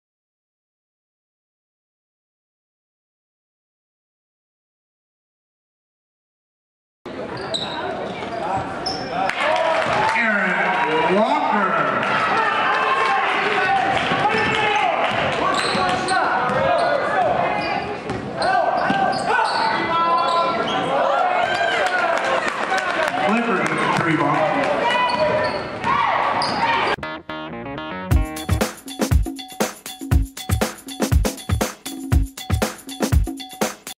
Davison JV Basketball @ Flushing
Travis Clifford three point bomb.